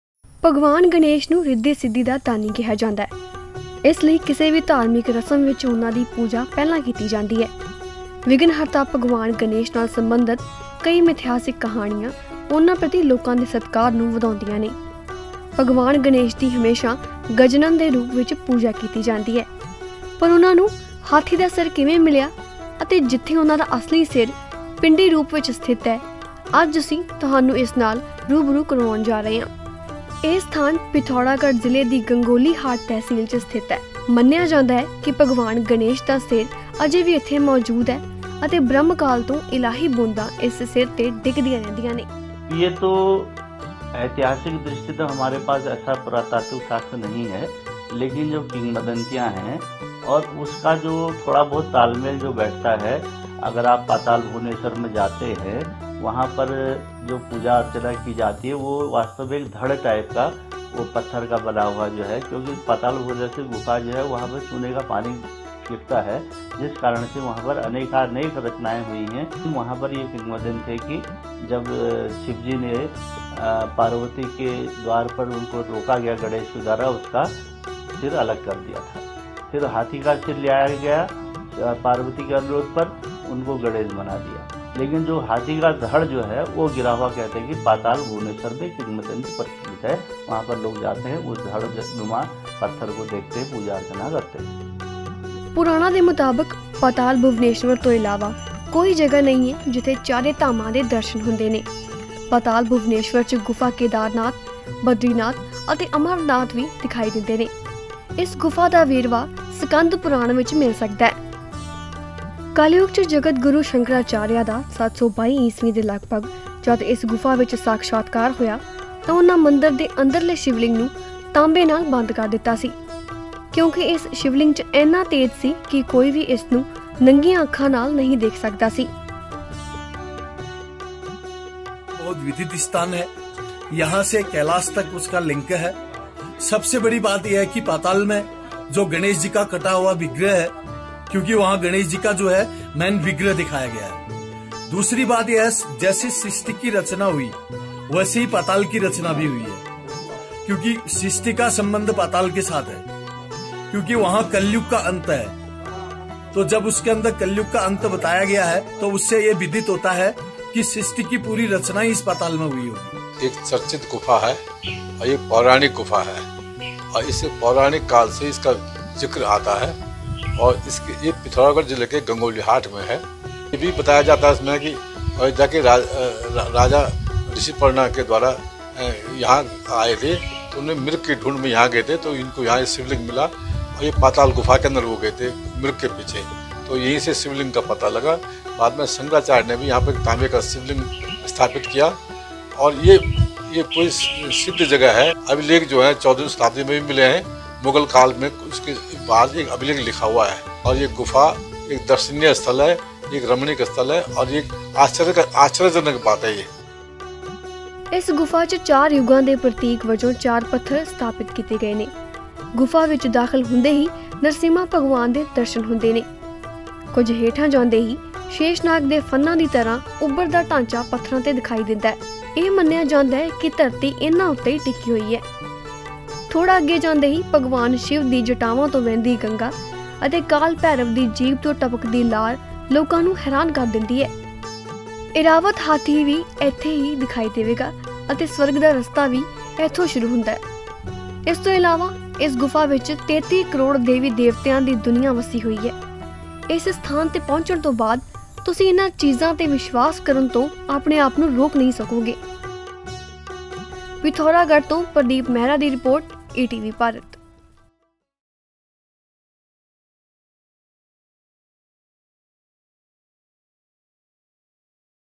ਪਾਤਾਲ ਭੁਵਨੇਸ਼ਵਰ | Patal Bhuvaneshwar | Ganesh Temple Uttarakhand | Special Story | ETV Bharat Punjab
ਰਿਧੀ-ਸਿੱਧੀ ਦੇ ਦਾਤੇ ਸ੍ਰੀ ਗਣੇਸ਼ ਦੀ ਪੂਜਾ ਕਿਸੇ ਵੀ ਧਾਰਮਿਕ ਰਸਮ ਵਿਚ ਕੀਤੀ ਜਾਂਦੀ ਹੈ।
ਆਓ ਜਾਣਦੇ ਹਾਂ ਭਗਵਾਨ ਗਣੇਸ਼ ਦੀ ਗਜਾਨਨ ਬਣਨ ਦੀ ਕਹਾਣੀ ਅਤੇ ਜਿੱਥੇ ਉਨ੍ਹਾਂ ਦਾ ਅਸਲ ਮਸਤਕ ਪਿੰਡੀ ਰੂਪ ਵਿੱਚ ਹੈ.